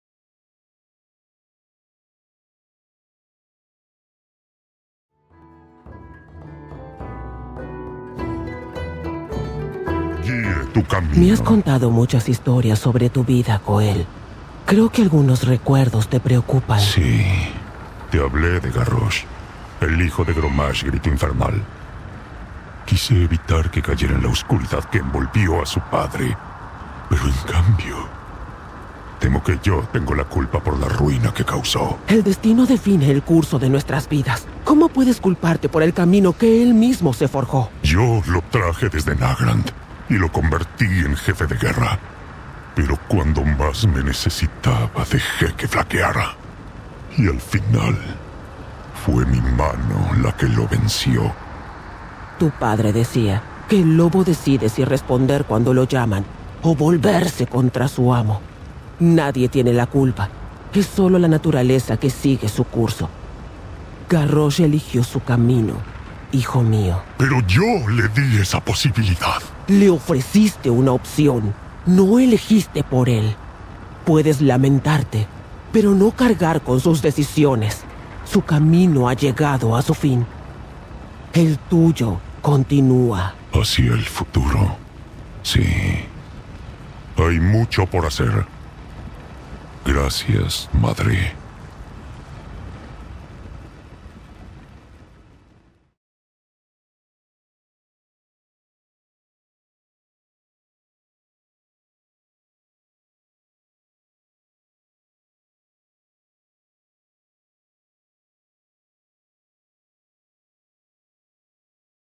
[LORE LATINO] Diálogo IN-GAME entre Draka y Thrall
Dakara y Thrall hablan acerda de la culpa que invade a su hijo